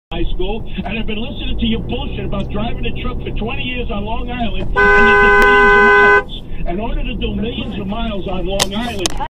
Left turn right in front of me.
Someone turns left in front of me.